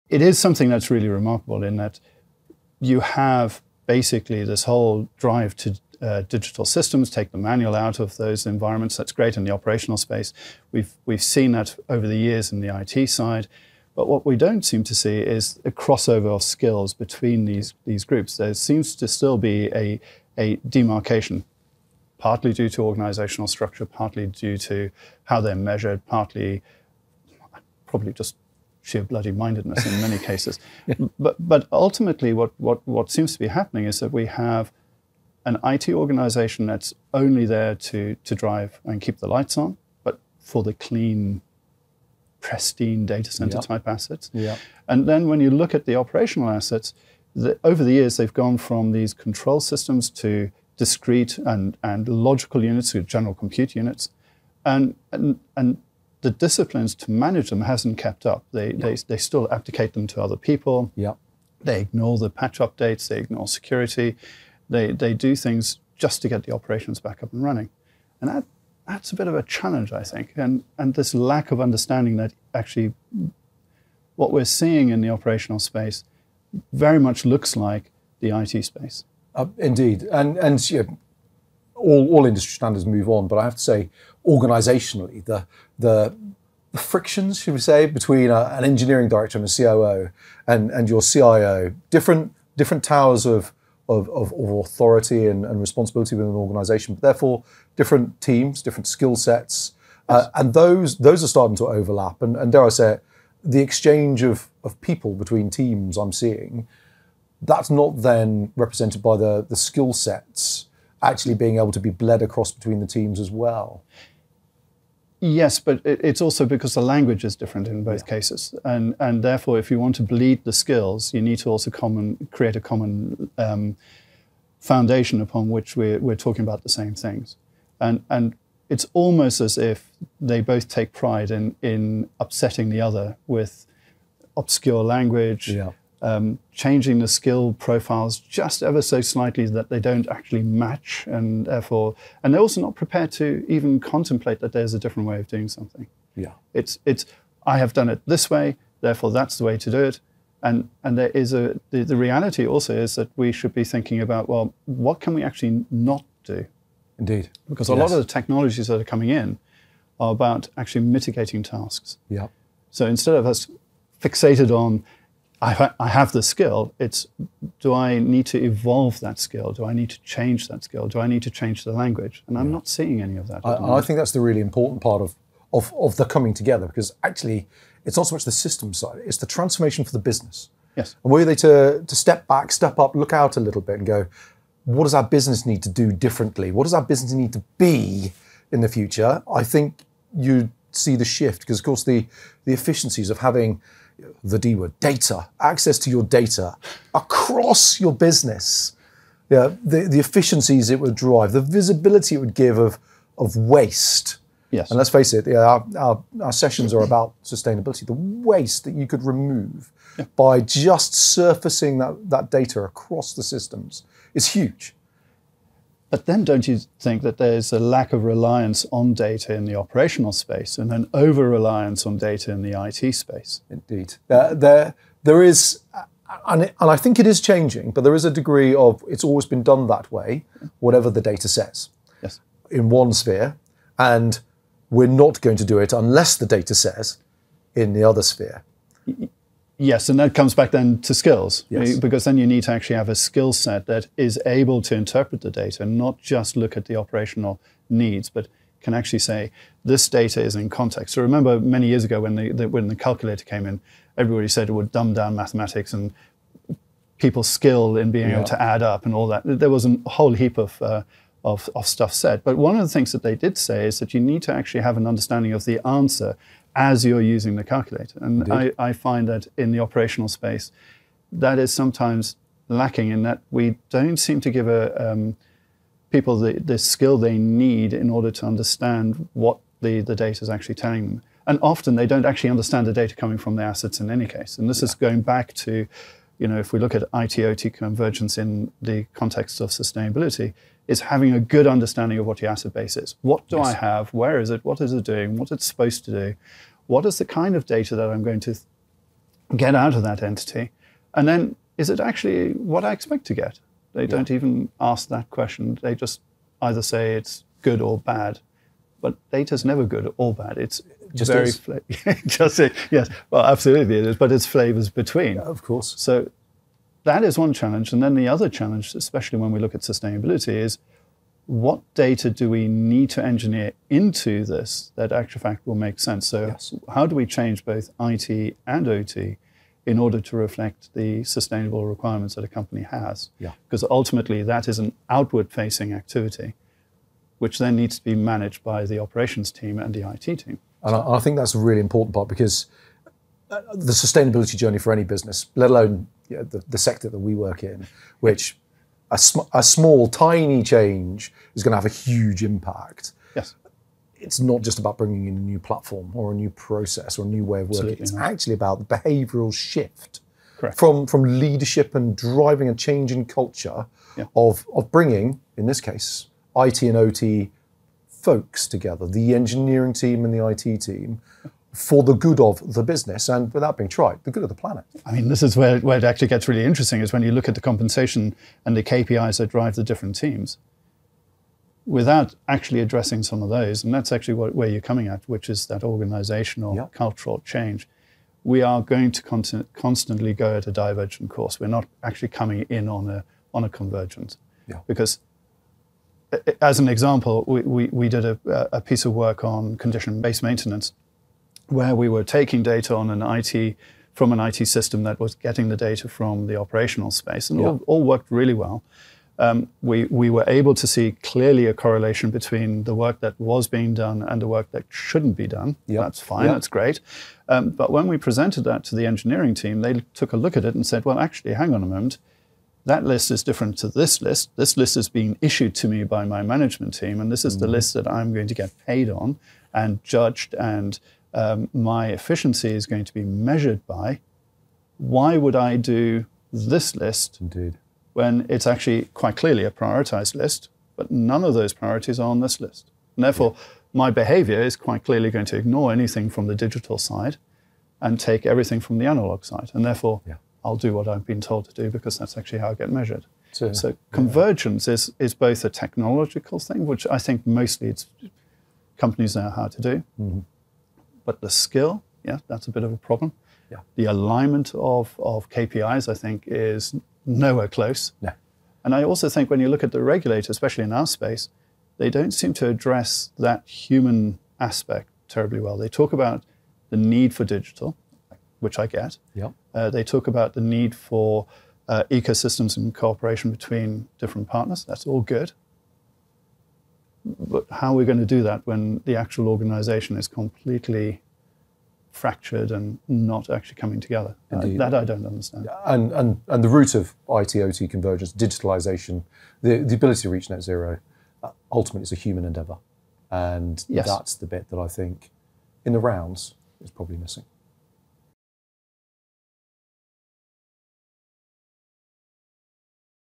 Breaking Down IT/OT Barriers: Bridging the Gap in the Utilities Sector
Register for A Deep Dive into the Utilities Sector’s Role in Shaping a Greener Planet:

Sven Strassburg (IBM) and Mark Hewett from (Capgemini) discuss the internal division between IT and operations which hinder progress and innovation within an organisation. The conversation covers their ideas and thoughts on what is causing the barriers to the business groups and the benefits which would come with improved collaboration.